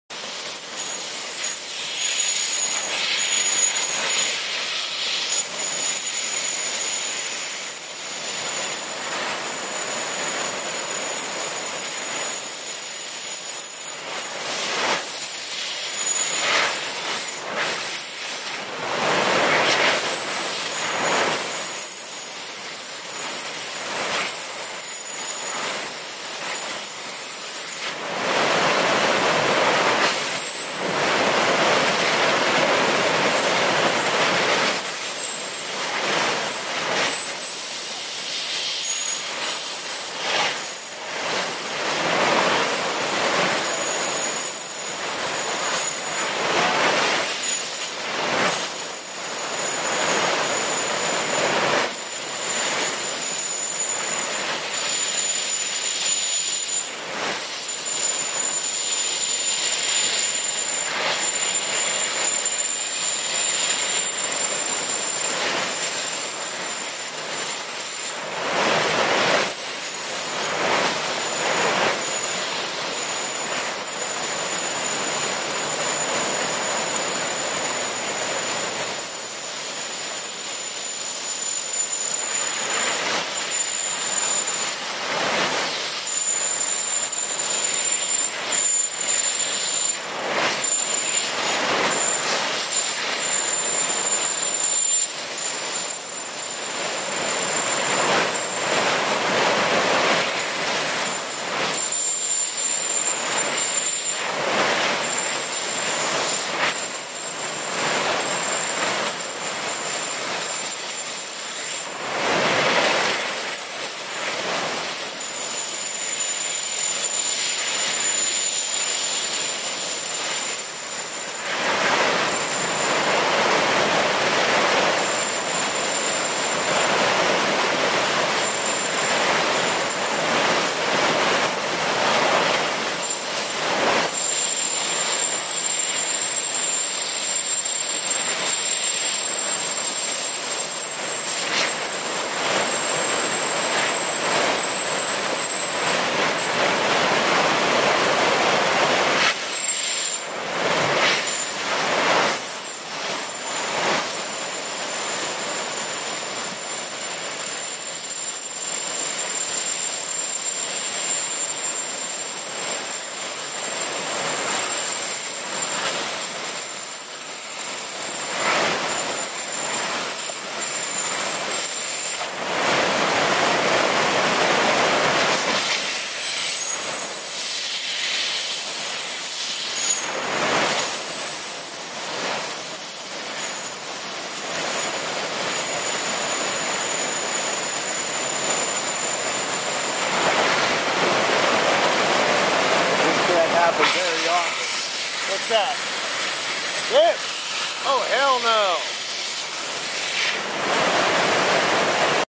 Gas Bottle leaking @ Stoney Creek dam
Stoney Creek Dam flood brought all kinds of stuff down stream to  the dam